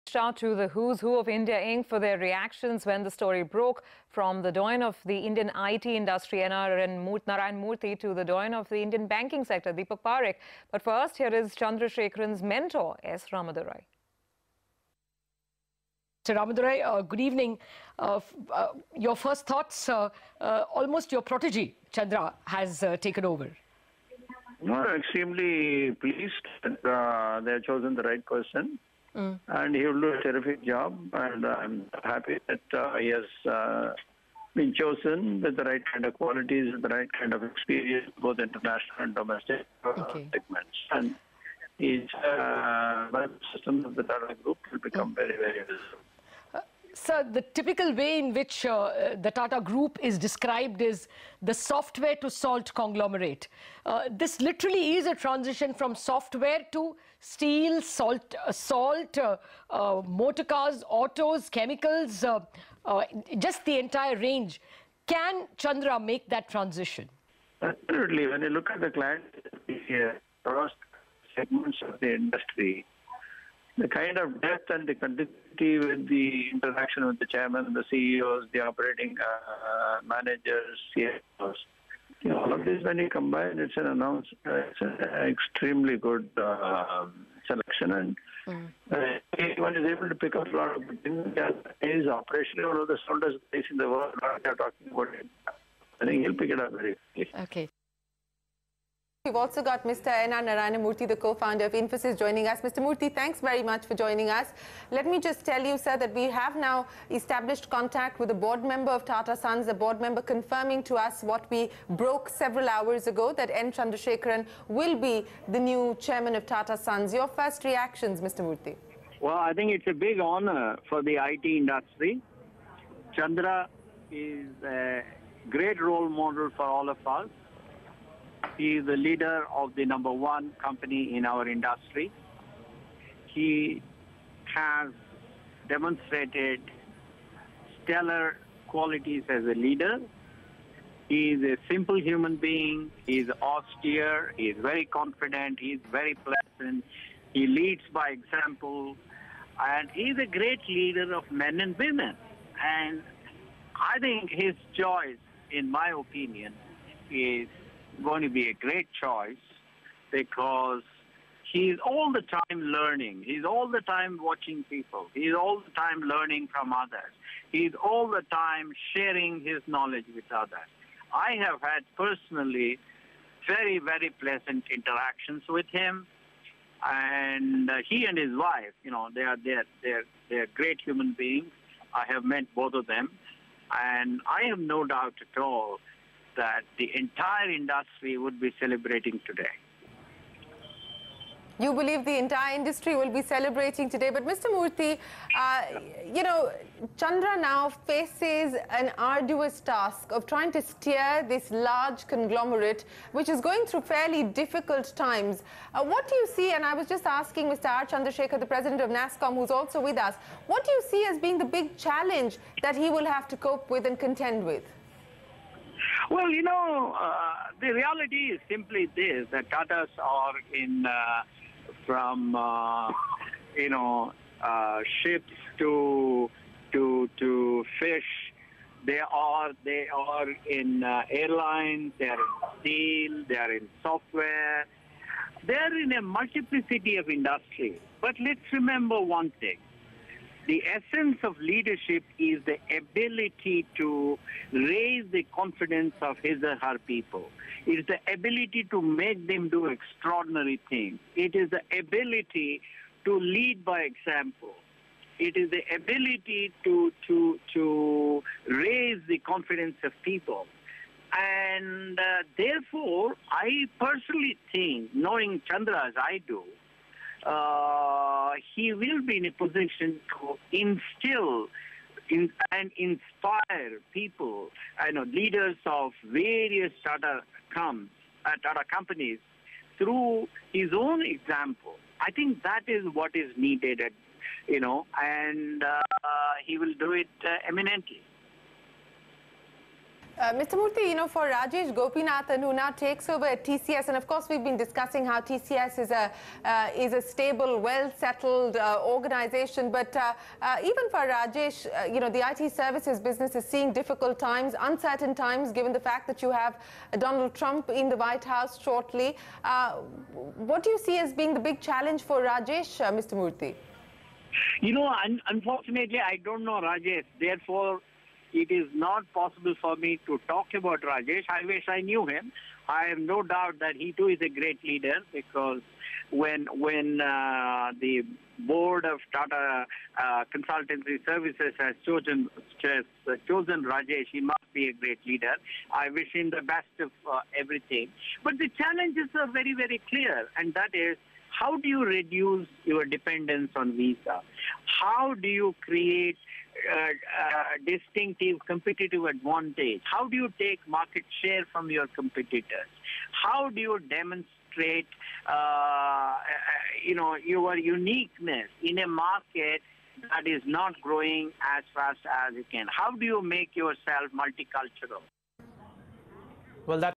N Chandrasekaran To Be Next Tata Sons Head
Here are the reactions of the Industry leaders upon the appointment of N Chandra as Tata Sons Chairman, starting with S Ramadorai & Narayana Murthy